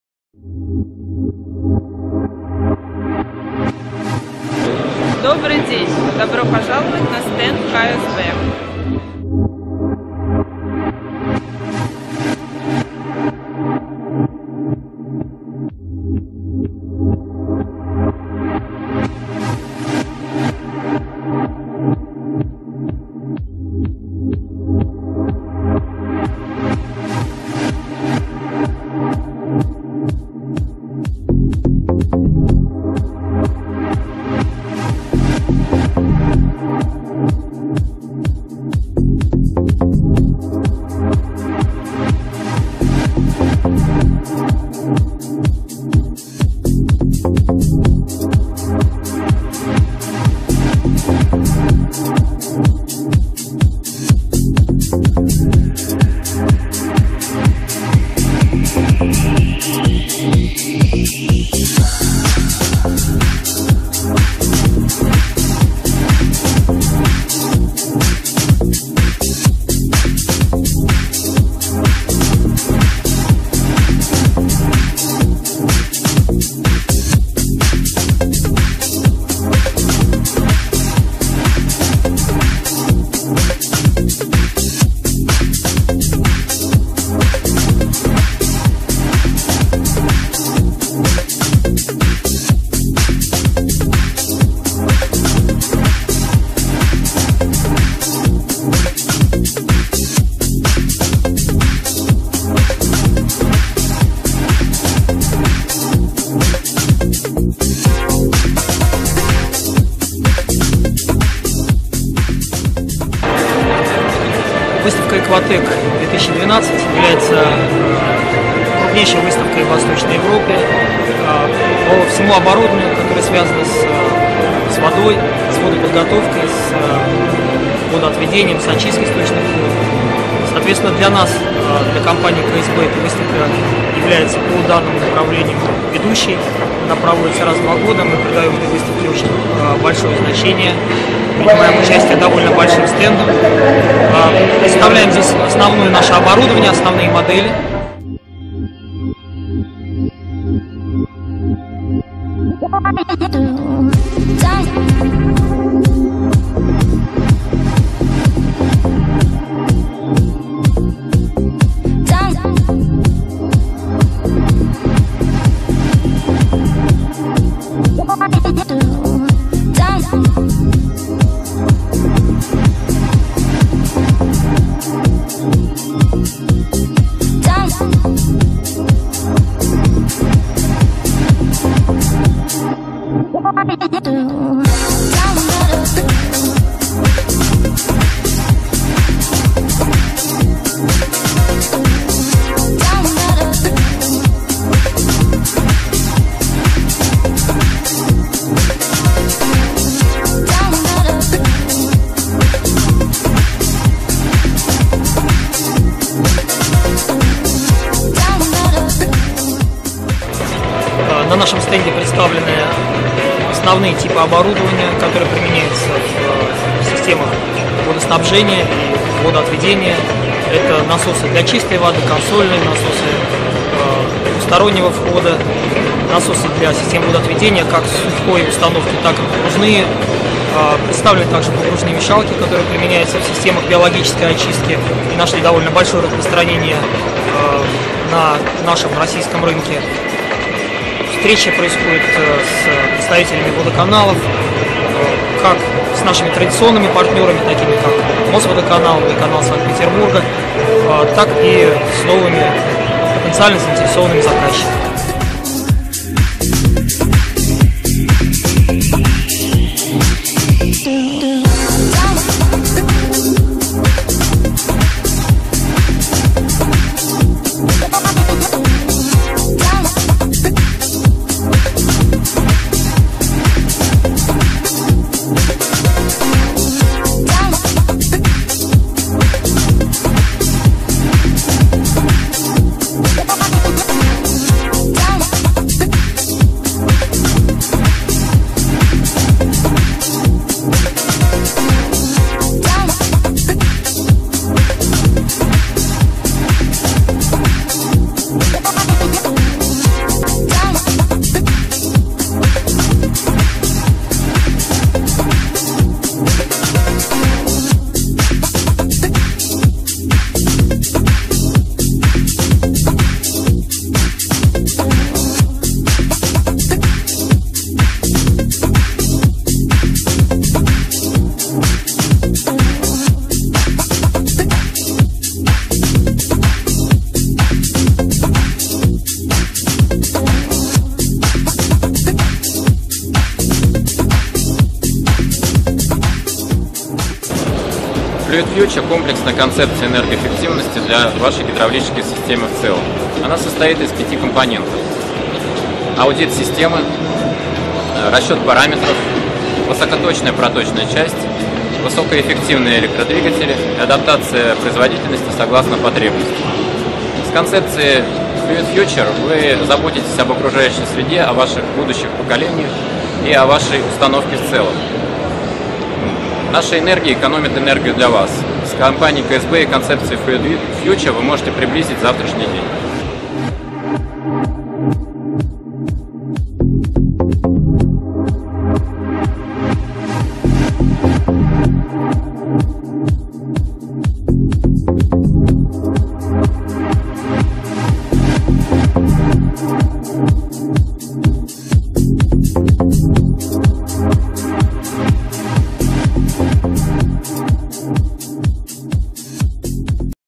KSB на выставке ECWATECH 2012
С 5 по 8 июня 2012 г. в Москве компания KSB представляла свой стенд на международной выставке ЭКВАТЭК по технологиям и оборудованию в сфере подготовки, снабжения, отвода и очистки воды. Потенциальным партнерам и заказчикам были показаны насосы (консольные, погружные, поверхностные, для чистой воды) и мешалки для систем биоочистки.